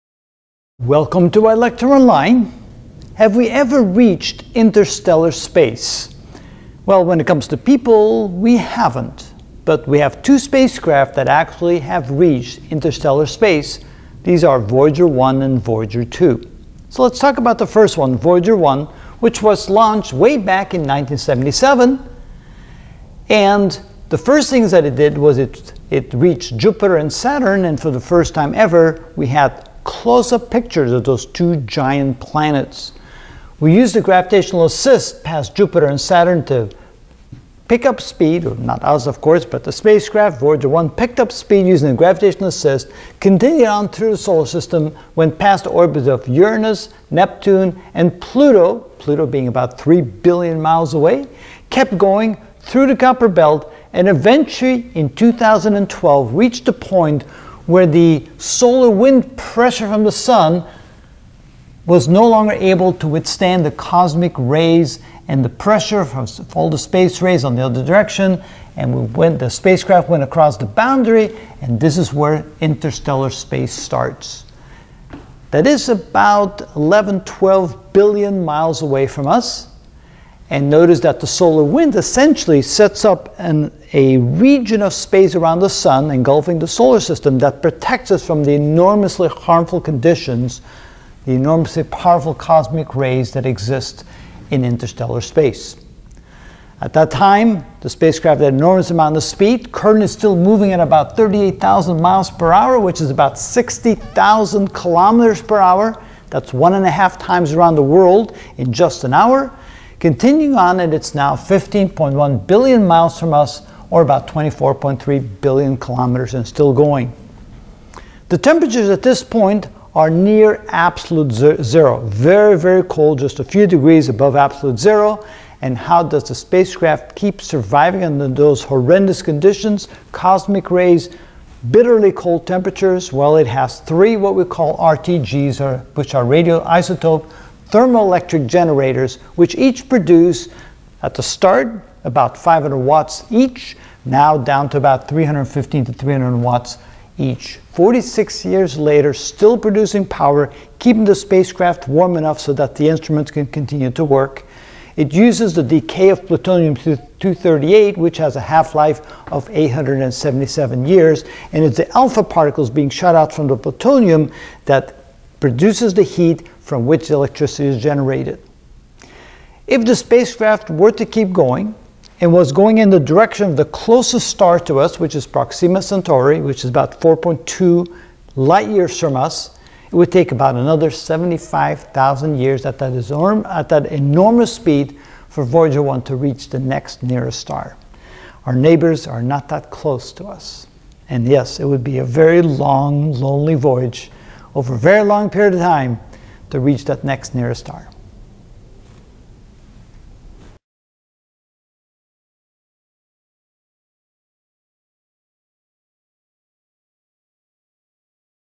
Can You Believe It? #46 The First Spacecraft To Reach Interstellar Space
We will learn about, how, and where the first spacecraft, Voyager 1, travelled to from Earth out of our Solar System, pass the Kuiper Belt, and is now in interstellar space.

Title search on other plateforms:  Can You Believe It? #45 Will The Sun Swallow Up The Earth

Title search on other plateforms:  Can You Believe It? #47 The Size Of The Miky Way Galaxy